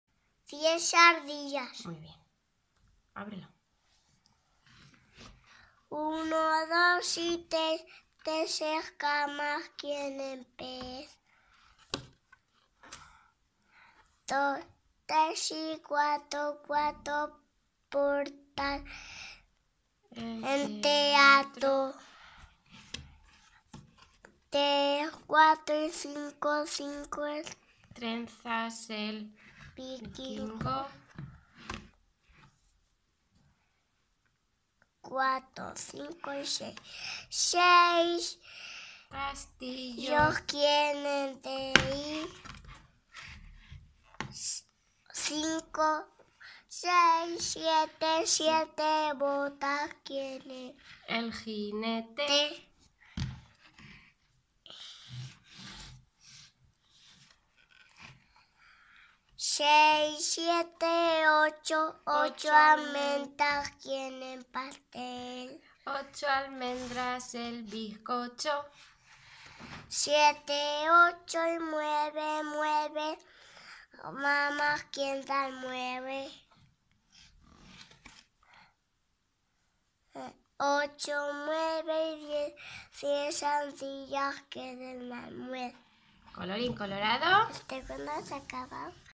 Cuento: Diez ardillas. Combel
Está narrado por mi hijo Didier, es de la editorial Combel y tienen detrás el código Qr para descargar la canción. Es una colección muy chula para niñ@s de infantil.